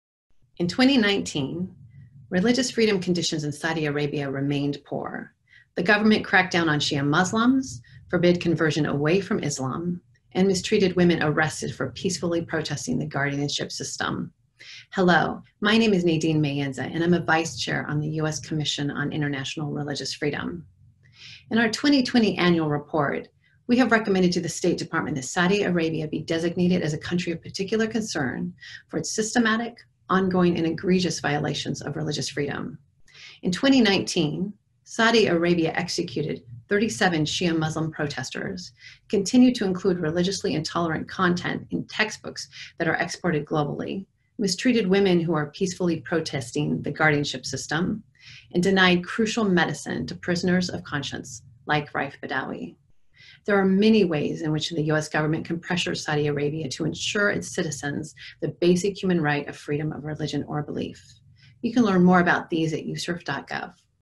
2020 Annual Report: Saudi Arabia
USCIRF Vice Chair Nadine Maenza describes poor religious freedom conditions in Saudi Arabia, such as the government crackdown on Shi’a Muslims, forbidden conversion away from Islam, and women arrested for peacefully protesting the guardianship system.

Learn more about religious freedom conditions in Saudi Arabia in the 2020 Annual Report, available in English and Arabic